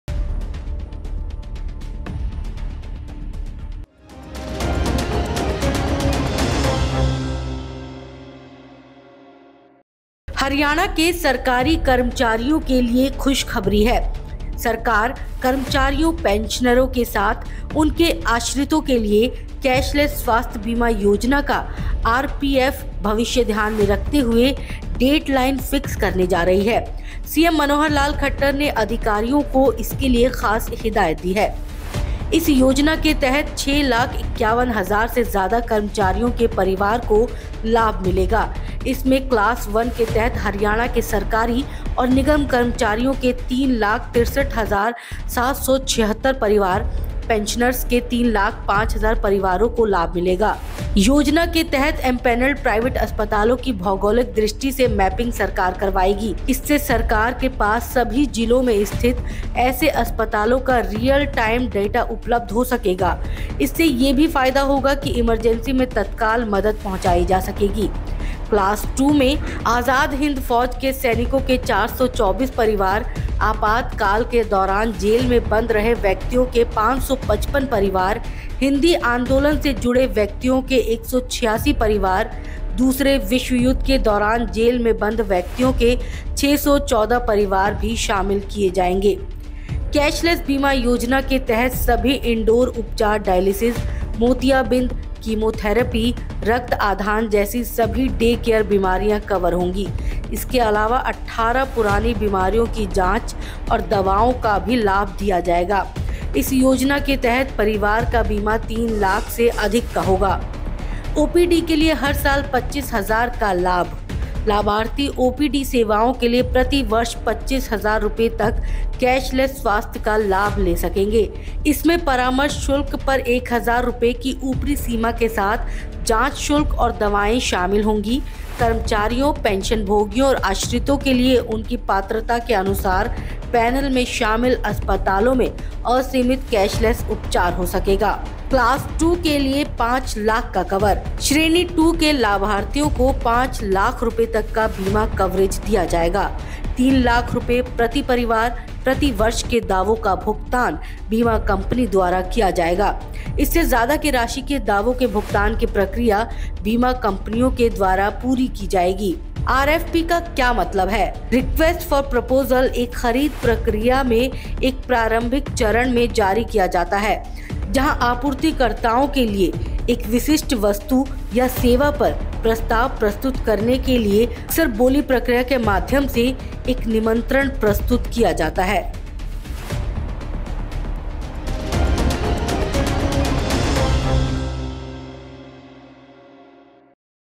हरियाणा के सरकारी कर्मचारियों के लिए खुशखबरी, अब हेल्थ कवर देगी सरकार | Haryana Aaj - Sabse Aage
हरियाणा के सरकारी कर्मचारियों के लिए खुशखबरी है। सरकार कर्मचारियों, पेंशनरों के साथ उनके आश्रितों के लिए कैशलेस स्वास्थ्य बीमा योजना का RFP भविष्य ध्यान में रखते हुए डेट लाइन फिक्स करने जा रही है। CM मनोहर लाल खट्‌टर ने अधिकारियों को इसके लिए खास हिदायत दी है।